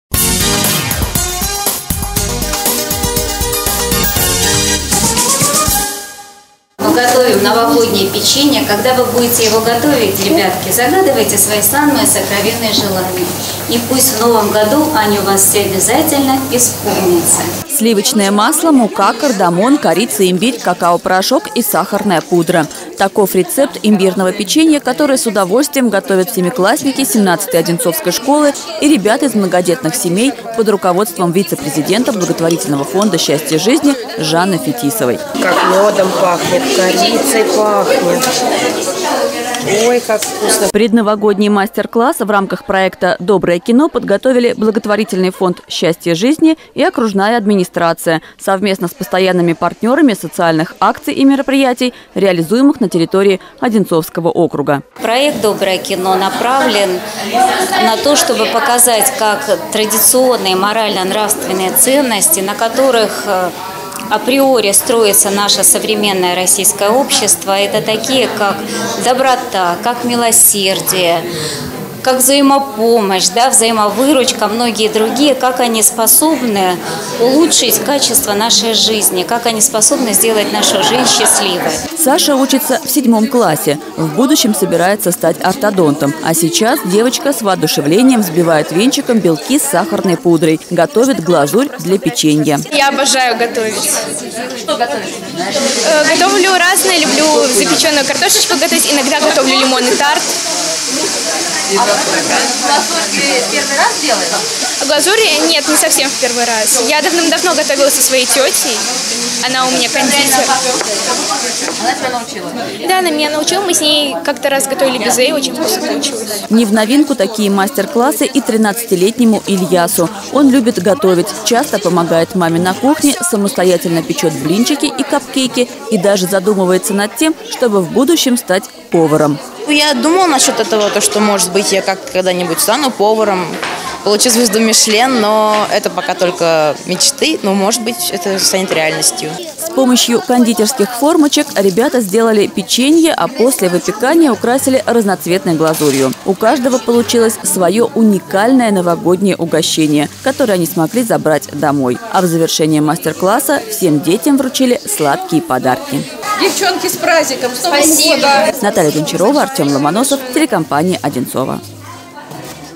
Новогодний мастер-класс
Семиклассники Одинцовской школы № 17 и дети из многодетных семей приняли участие в мастер-классе по приготовлению новогоднего печенья от благотворительного фонда «Счастье жизни».